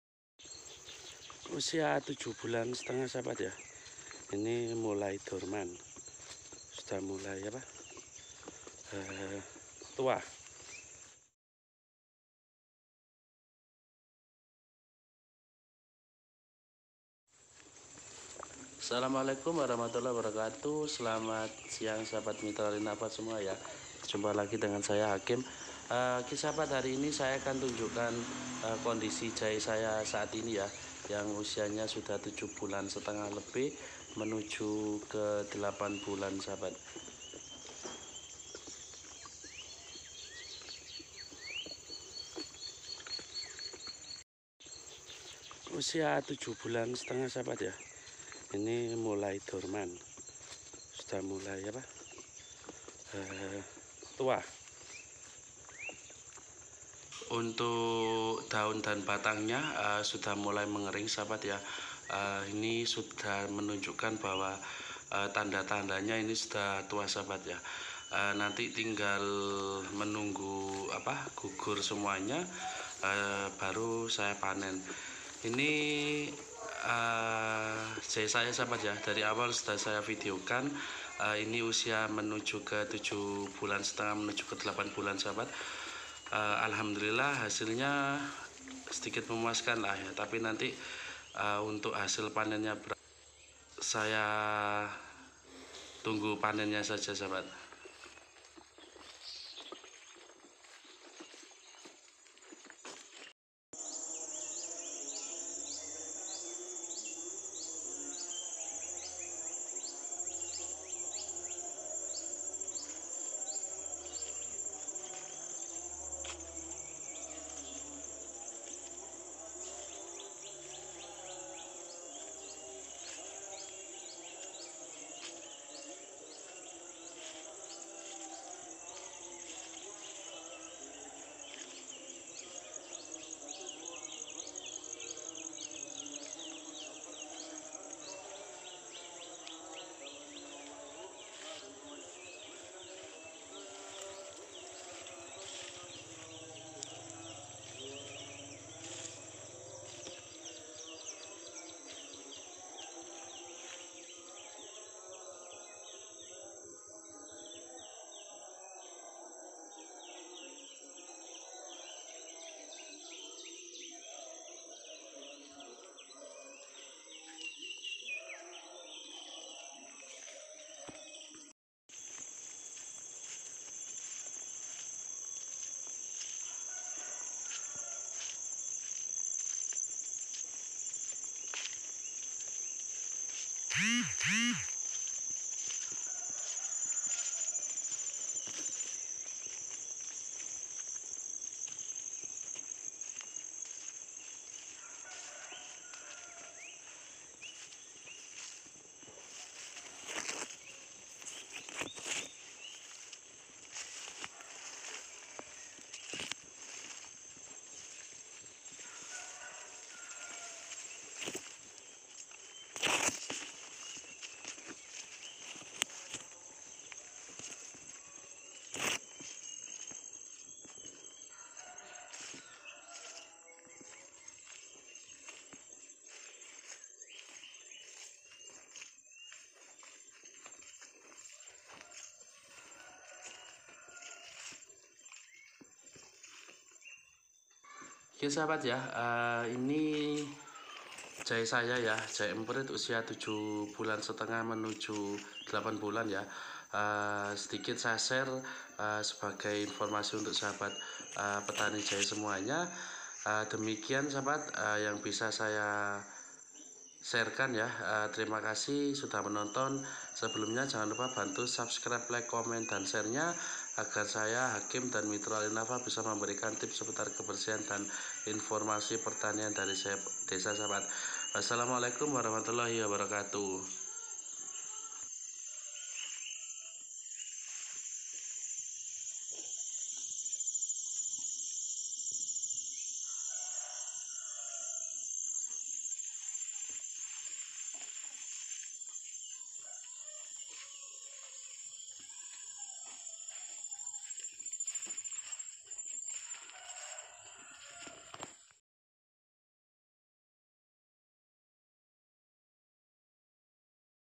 JAHE EMPRIT USIA MENUJU 8 BULAN
up date kondisi jahe menuju 8 bulan.  daun batangulai gugur menandakan jahe siap panen.